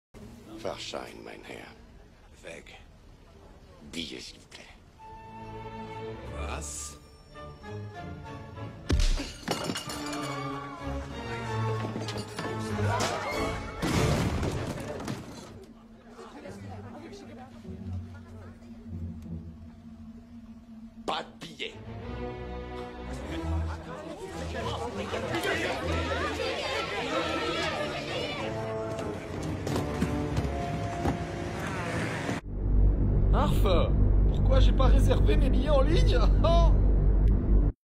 Billet en ligne World of Geek
Ne faites pas comme lui, prenez vos billets en ligne pour être sur d'avoir de la place au salon World of Geek de Fréjus.